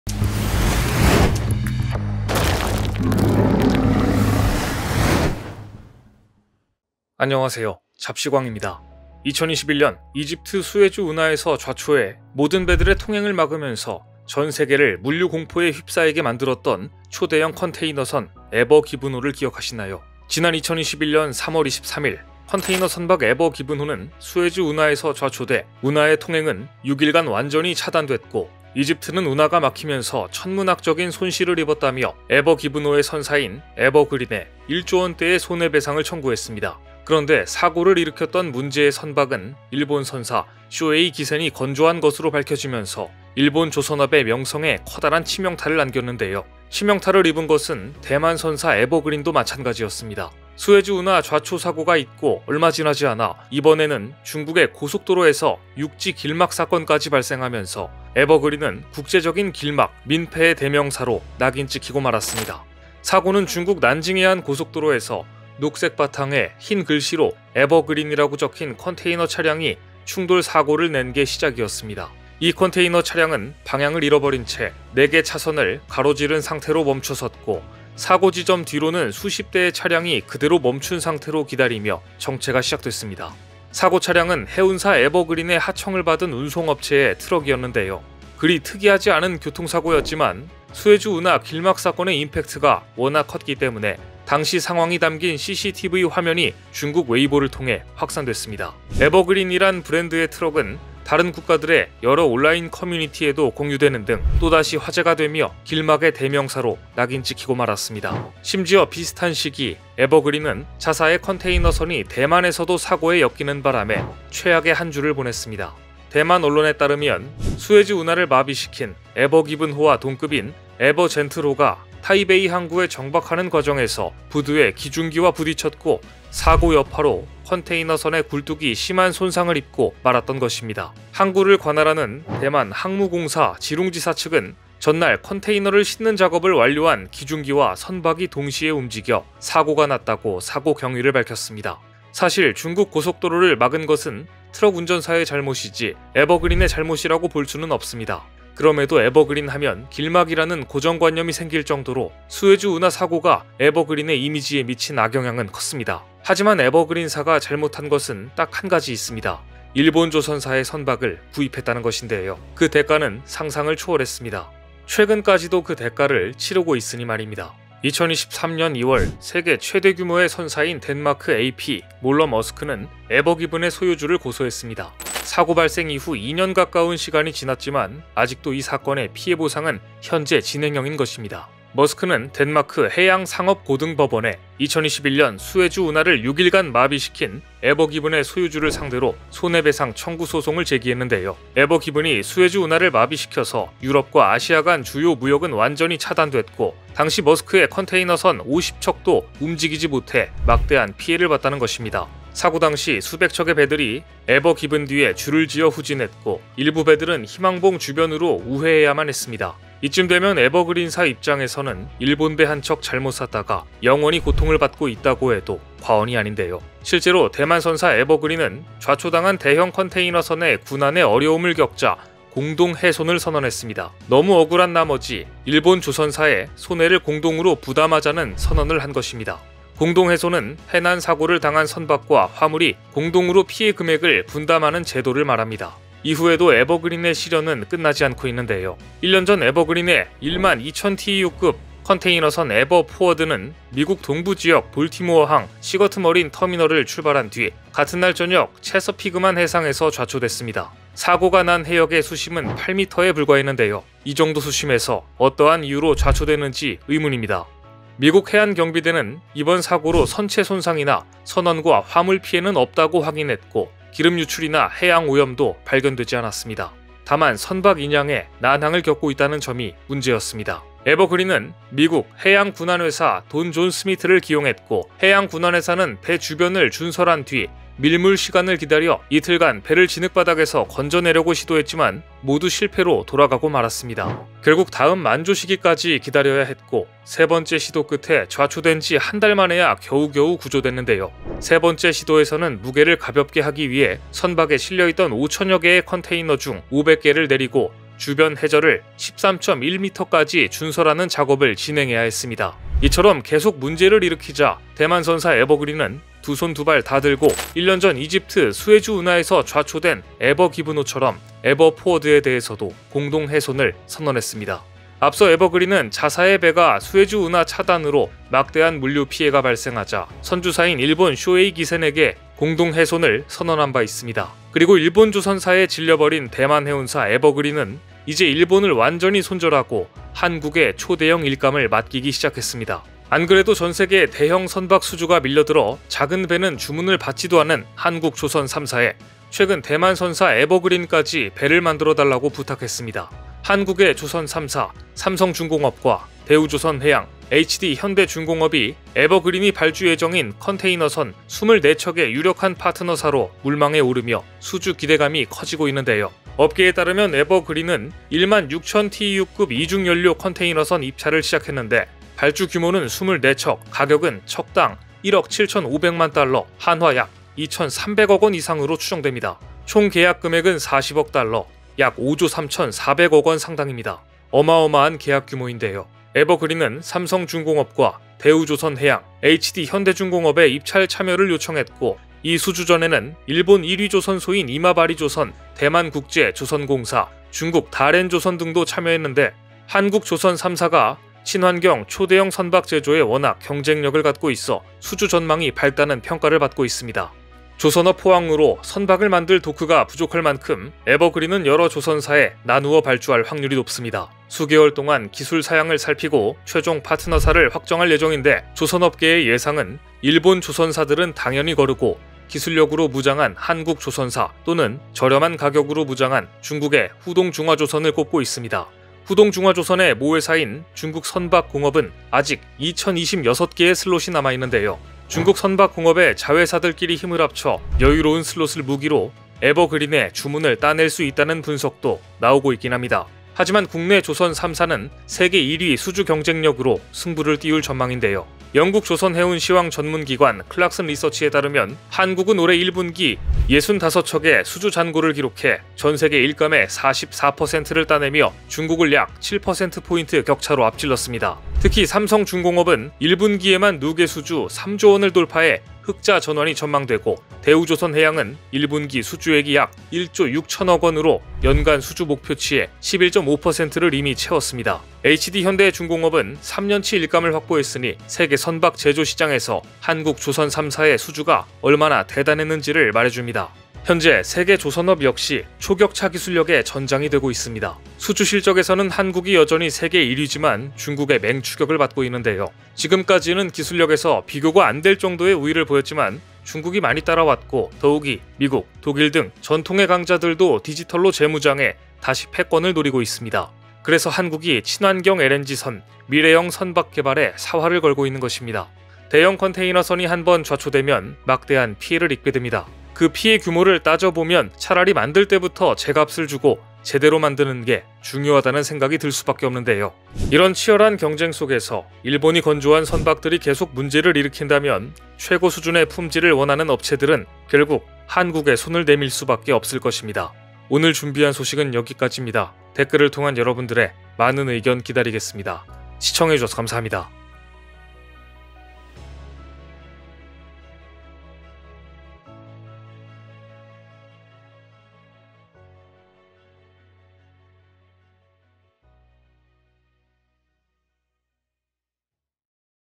단독! 대만 에버그린 또다시 길막사건 발생 | 일본에 추가소송 피해보상 금액 계산불가 / 일본배에 질려버린 에버그린사 5조들여서 한국산으로 다 바꾼다
채널 멤버쉽에 가입하여 잡식왕을 응원해주세요.
멤버쉽 비용은 전부 더 좋은 컨텐츠를 제작을 위해 사용됩니다.